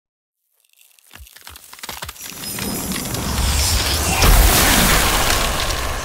Cherry Freeze Hydration
Perfectly capturing the essence of crisp red cherries with a citrusy frost twist. It’s cold-activated bottle transforms from white to blue when chilled, adding interactive fun to hydration ❄️